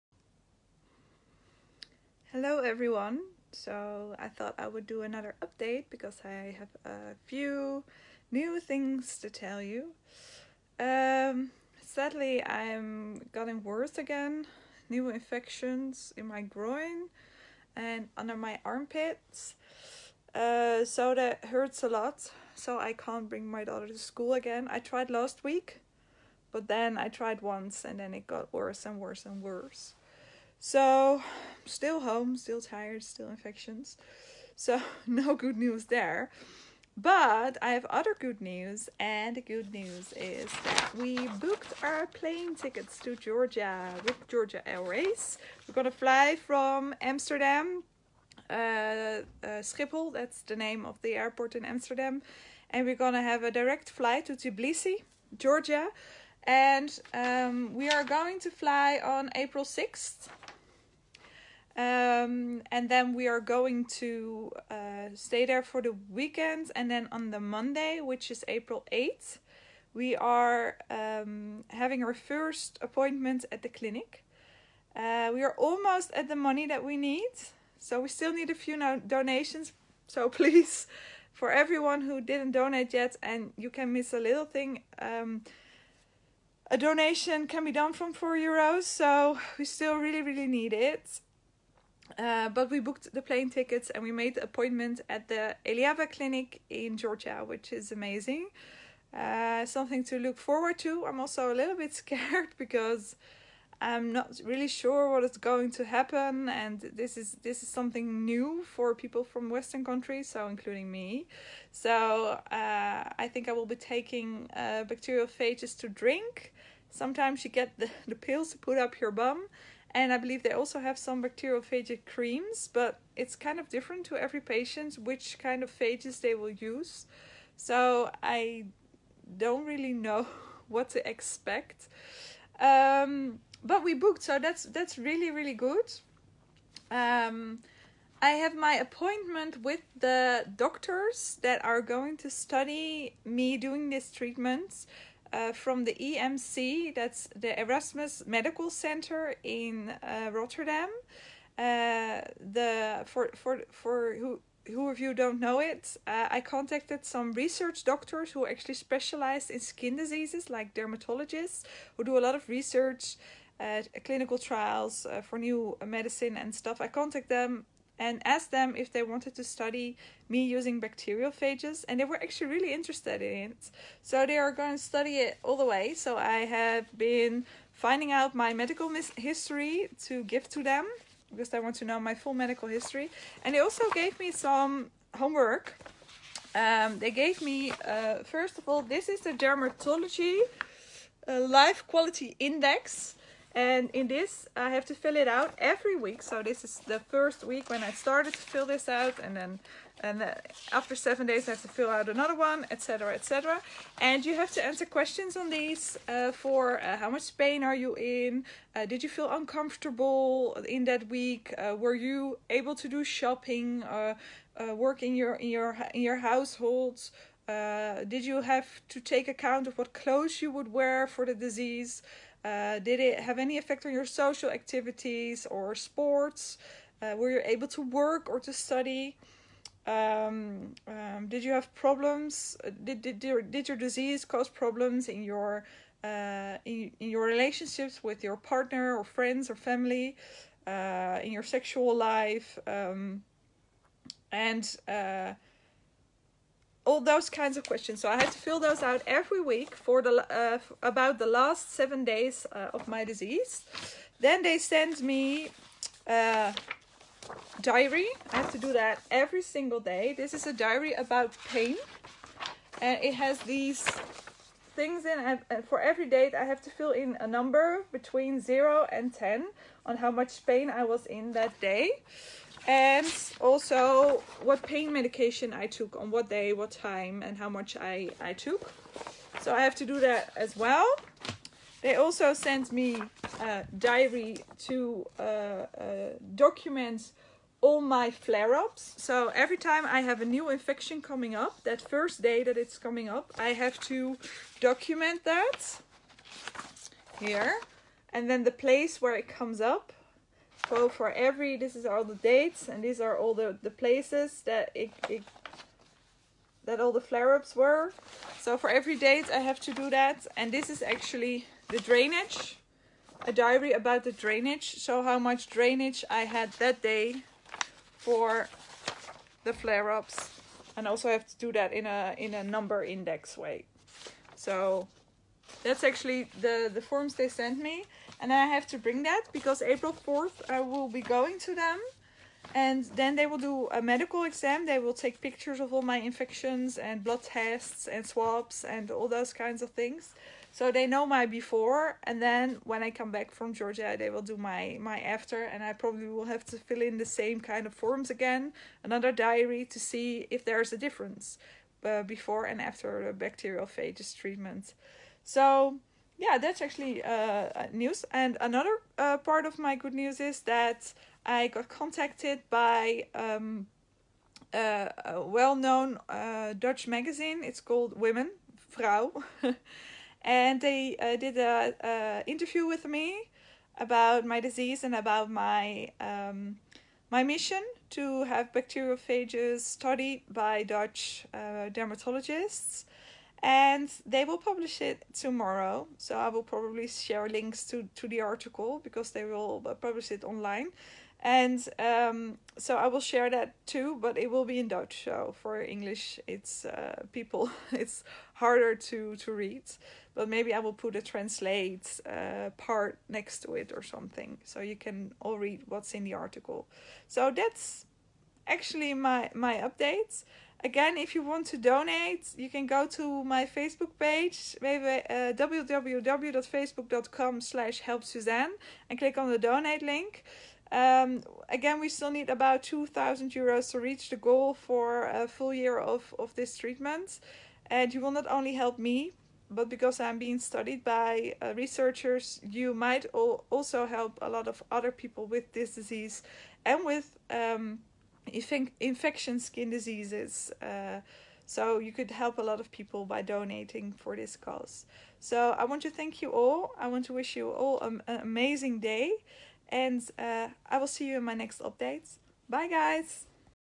Episode 8 - The EMC study on my bacteriophage treatment / Hidradenitis Suppurativa
Update on the Erasmus Medical Center who is studying me before and during my bacteriophage treatment.
We booked plane tickets and made our first appointment at the Eliava clinic